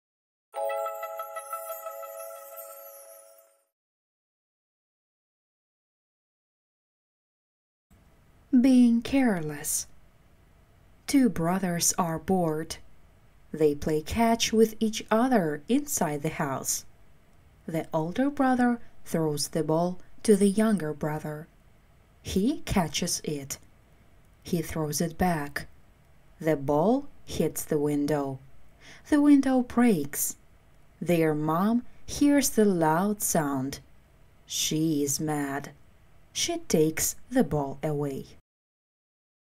#72 Being careless /// Short stories /// LISTEN & ENJOY Beginner A1
Я читаю короткие рассказы, а вы слушаете и пробуете находить знакомые слова. Текст специально не показываю, чтобы не отвлекал.

Рассказ можно найти в прекрасном пособии "Start reading for children: 500 short stories - beginner"

I read short stories and you listen and try to find familiar words. I do not show the text on purpose, so as not to distract.

The story can be found in the excellent manual "Start reading for children: 500 short stories - beginner"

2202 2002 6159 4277 Sberbank